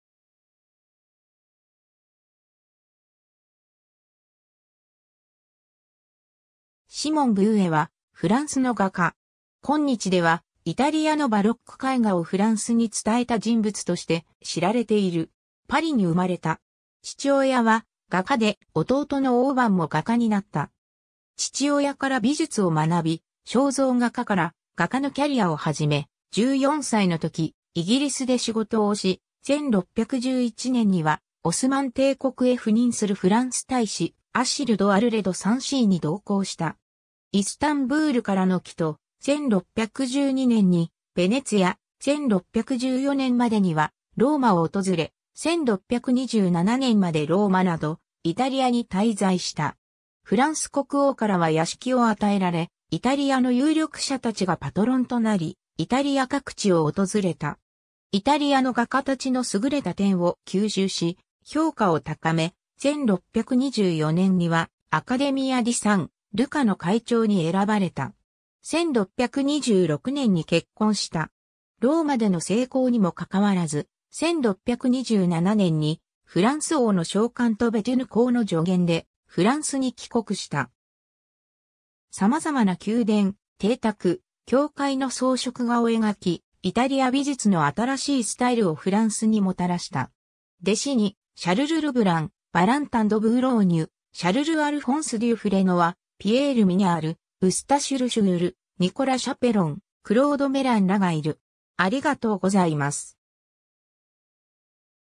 シモン・ヴーエ
フランス・バロックの画家
フランスのタペストリー・アーティスト
シモン・ヴーエ（Simon Vouet、フランス語: [vwɛ]、1590年1月9日 - 1649年6月30日）は、フランスの画家。
今日ではイタリアのバロック絵画をフランスに伝えた人物として知られている。
 パリに生まれた。
父親は画家で弟のオーバン（Aubin Vouet, 1595年 – 1641年）も画家になった。
 父親から美術を学び、肖像画家から画家のキャリアを始め、14歳の時イギリスで仕事をし、1611年にはオスマン帝国へ赴任するフランス大使アシル・ド・アルレ・ド・サンシーに同行した。
イスタンブールからの帰途、1612年にヴェネツィア、1614年までにはローマを訪れ、1627年までローマなどイタリアに滞在した。
 フランス国王からは邸を与えられ、イタリアの有力者たちがパトロンとなり、イタリア各地を訪れた。
イタリアの画家たちの優れた点を吸収し評価を高め、1624年にはアカデミア・ディ・サン・ルカの会長に選ばれた。
1626年に結婚した。
 ローマでの成功にもかかわらず、1627年にフランス王の召還とベテュヌ公（Duc de Béthunes）の助言でフランスに帰国した。
さまざまな宮殿、邸宅、教会の装飾画を描き、イタリア美術の新しいスタイルをフランスにもたらした。
 弟子にシャルル・ルブラン、ヴァランタン・ド・ブーローニュ、シャルル＝アルフォンス・デュ・フレノワ、ピエール・ミニャール、ウスタシュ・ル・シュウール、ニコラ・シャペロン、クロード・メランらがいる。